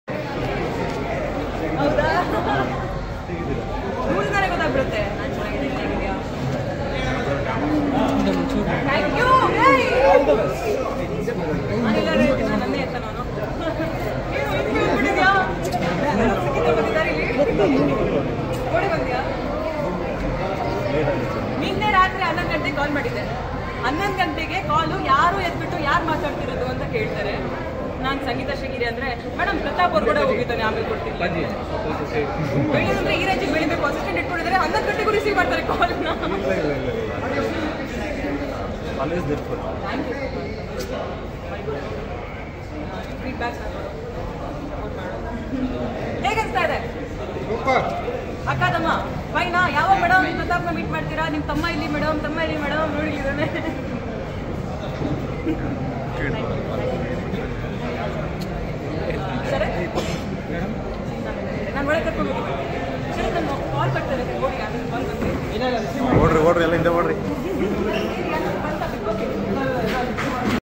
🔴ಸಂಗೀತಾನ ಎತ್ತಿಕೊಂಡ ಪ್ರತಾಪ್🙏-Marigold premier show |sangeetha sringeri new movie
Drone liftingsangeetha at marrigold premier show.